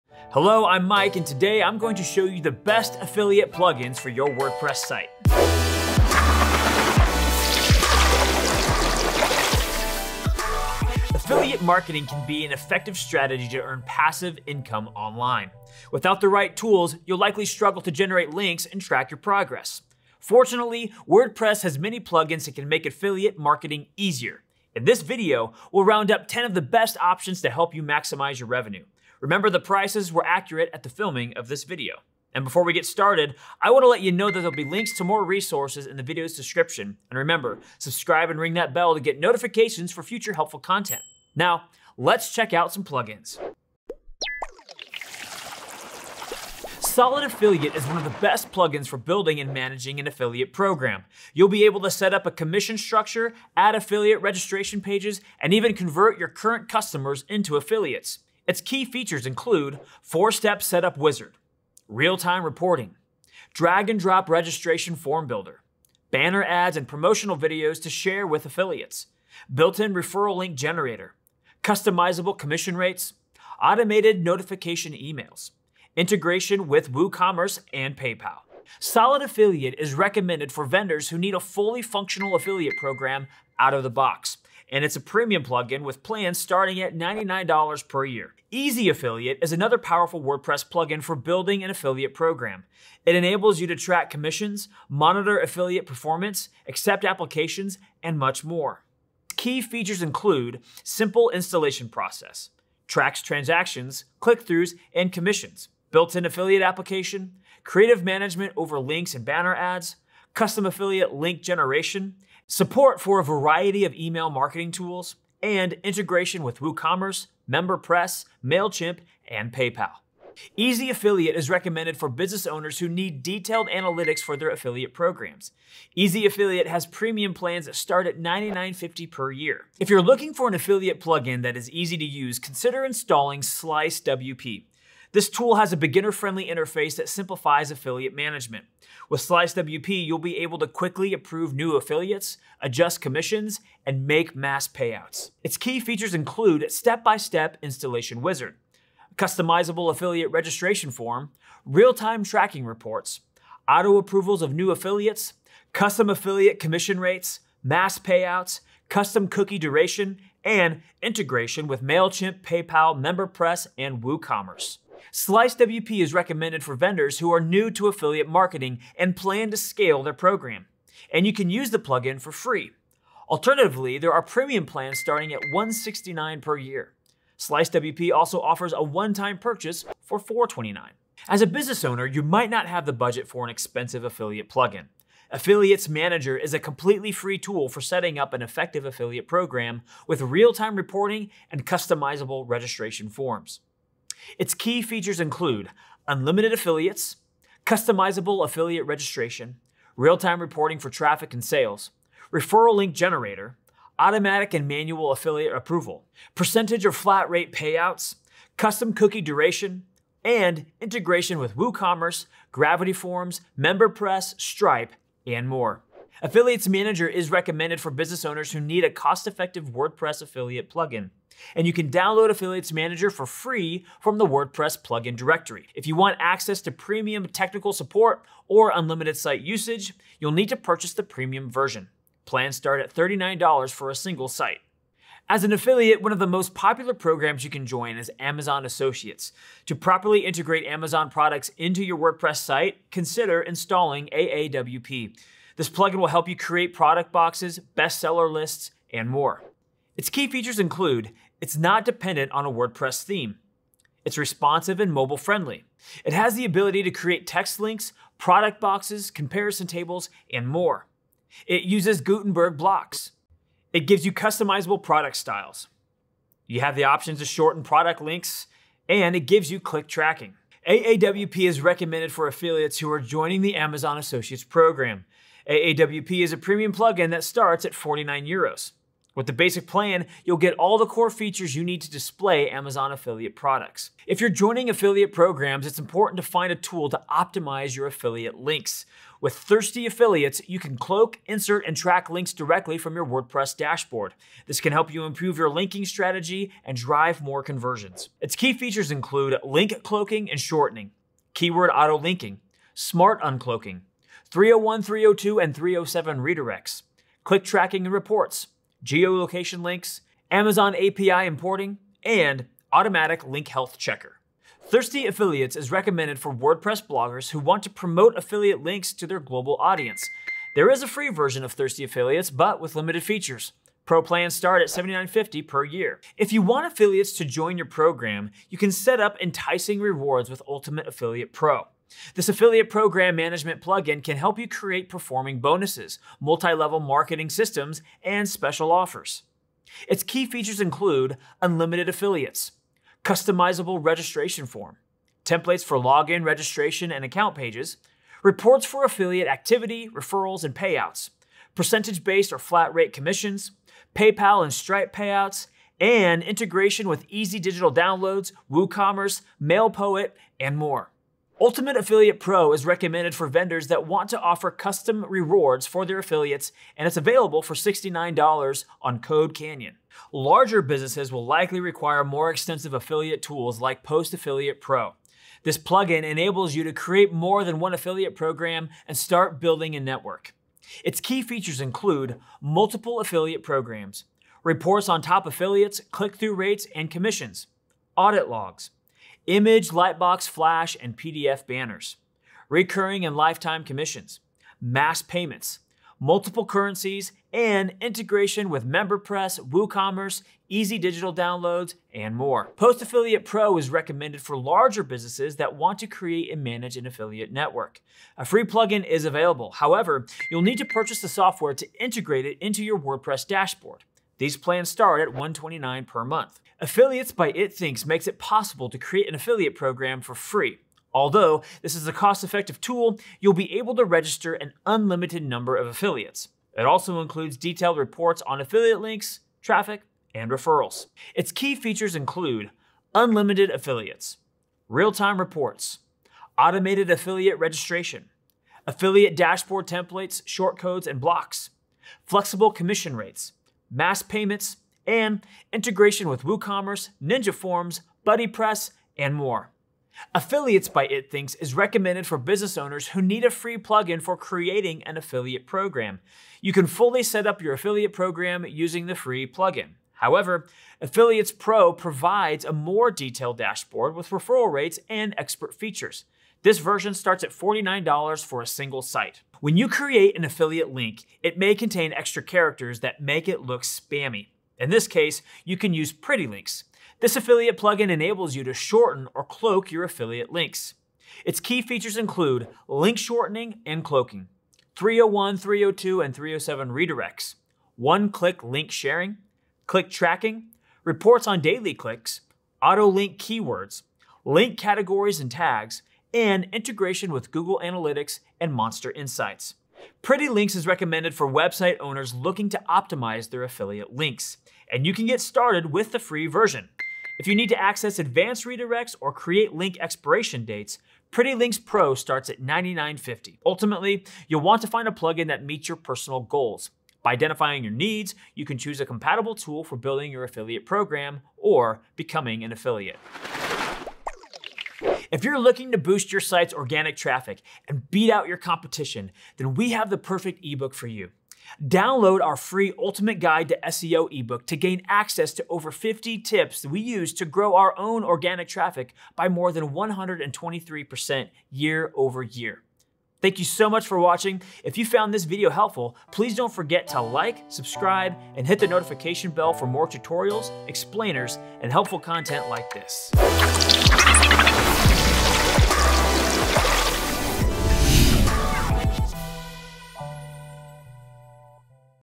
10 Best WordPress Affiliate Plugins to Sell More in 2023
Want to get started on affiliate marketing but need help figuring out where to start? 😣 Find the answers you need in this guide 💪

Affiliate marketing can be an effective strategy to earn passive income online. However, you may not know how to become an affiliate or launch your own affiliate program. Without the right tools, you’ll likely struggle to generate links and track your progress.

Fortunately, WordPress has many plugins that can make affiliate marketing easier. If you’re a content creator, these tools will help you optimize your affiliate links. Meanwhile, if you run an online business, you can use plugins to create a successful affiliate program.

In this video, we’ll explain what affiliate marketing is and what to look for in a WordPress affiliate plugin. Then, we’ll round up 10 of the best options to help you maximize your revenue. Let’s get started!

🕘Timestamps

0:00 Intro
0:50 Solid Affiliate
1:30 Easy Affiliate
2:16 SliceWP
3:07 Affiliates Manager
4:00 AAWP
4:49 ThirstyAffiliates
5:32 Ultimate Affiliate Pro
6:13 Post Affiliate Pro
6:59 Affiliates by itthinx
7:57 Pretty Links

📚 Resources